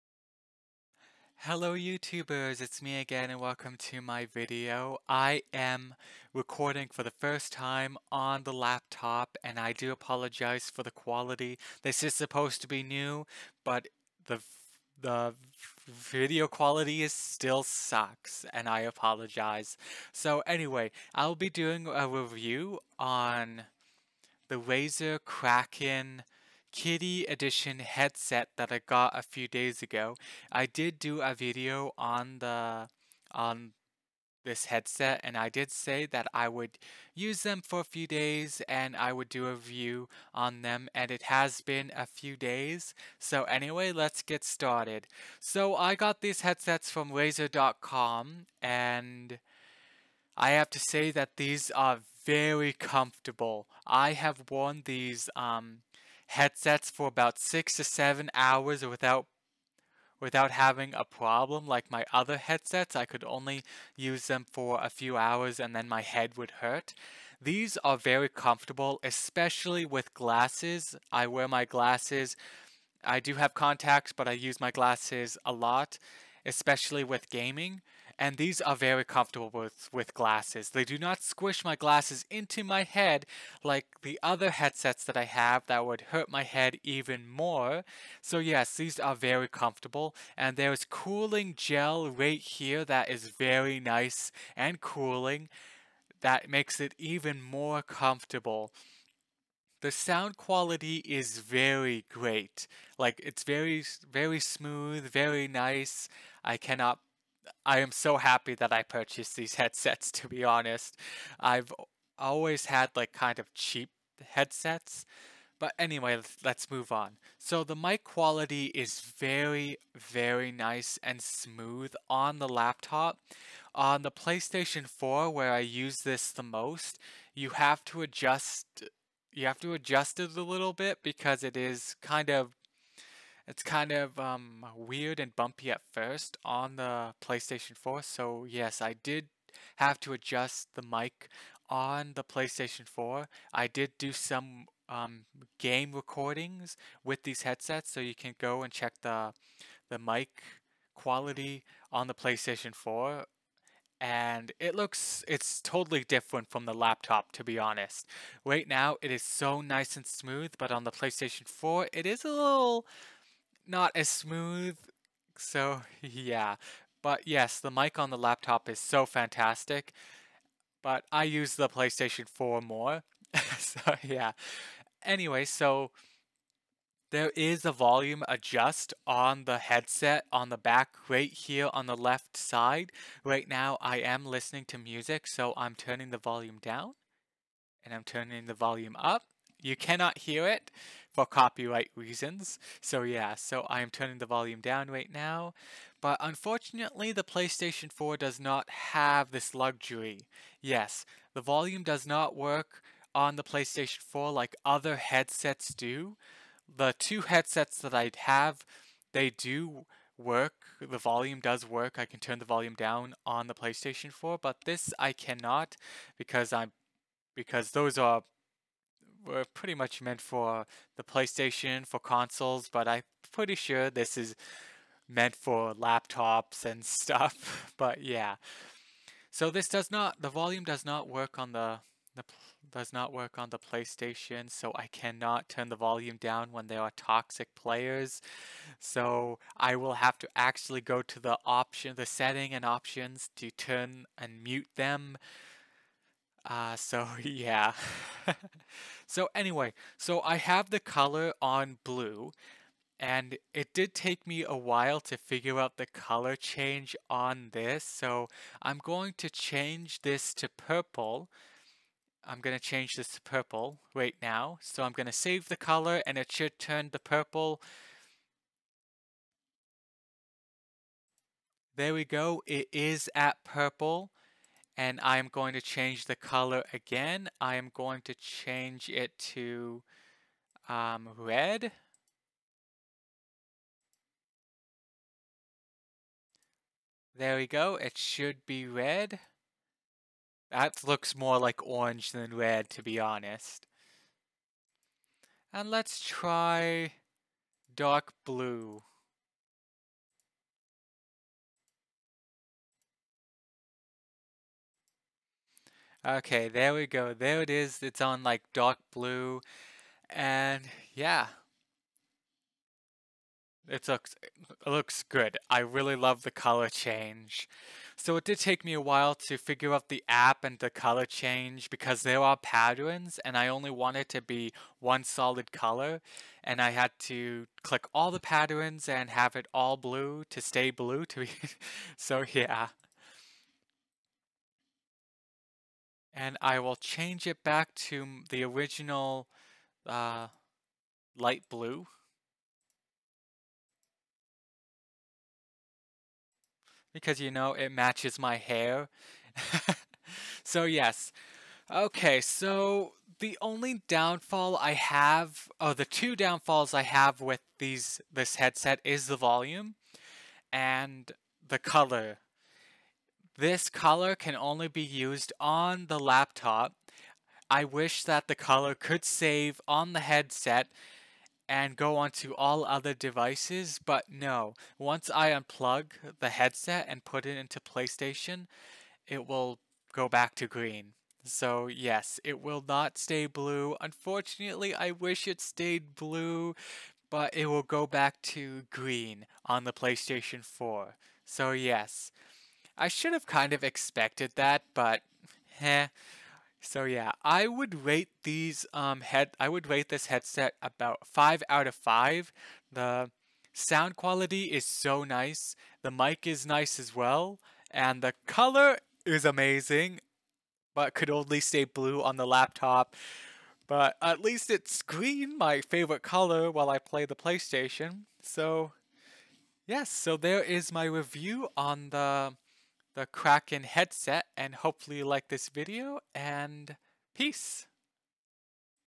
Razer Kraken Kitty Headset Review
Hello so i'm doing a review on these Headsets that i got and i just love them. and im sorry for the Video Quality my first time using.